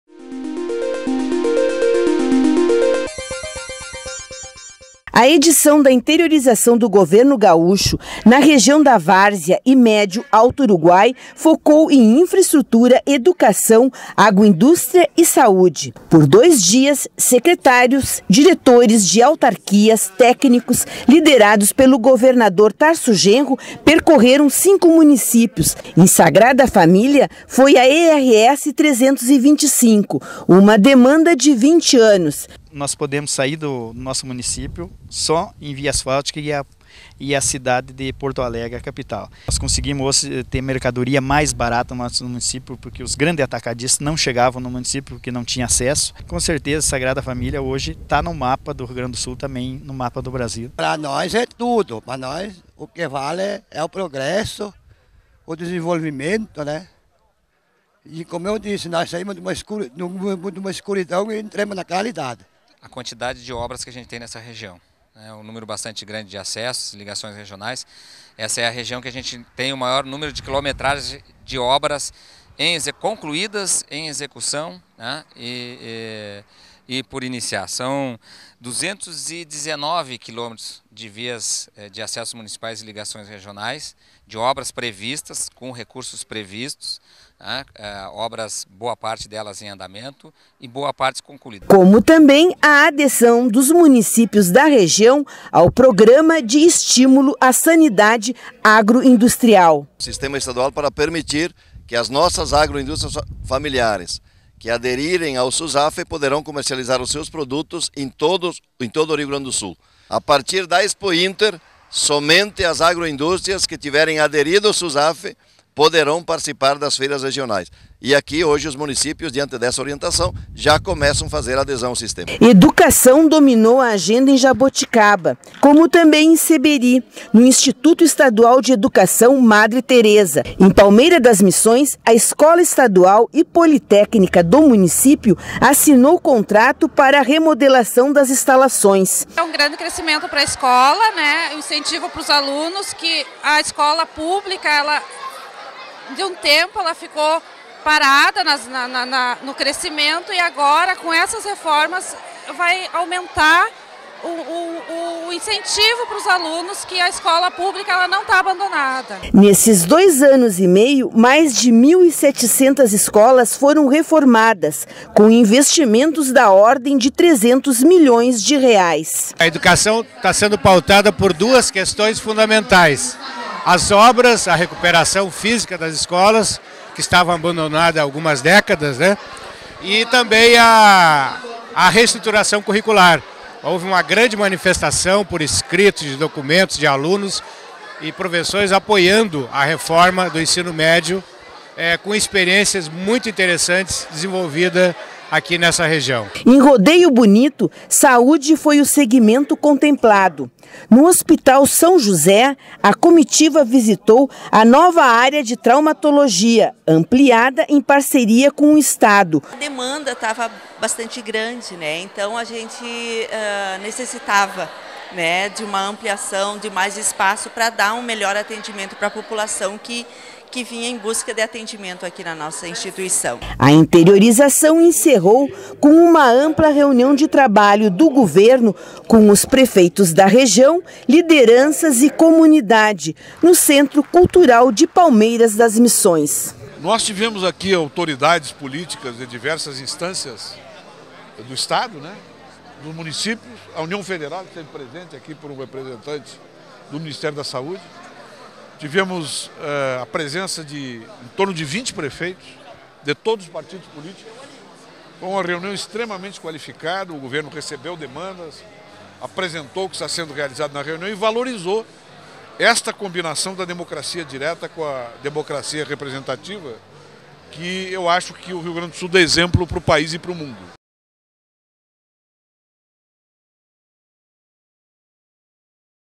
Educação, saúde e agricultura pautam Interiorização no Médio Alto Uruguai
Uma reunião de trabalho entre o governador Tarso Genro, secretários de Estado, prefeitos da região, vereadores e lideranças de movimentos sociais no município de Palmeira das Missões, encerrou a Interiorização do Governo do Estado, que durante a quinta e sexta-feira (22 e 23) percorreu cinco municípios das regiões Rio da Várzea e Médio Alto Uruguai. As pautas foram baseadas em infraestrutura, saúde, educação, desenvolvimento e agroindústria familiar.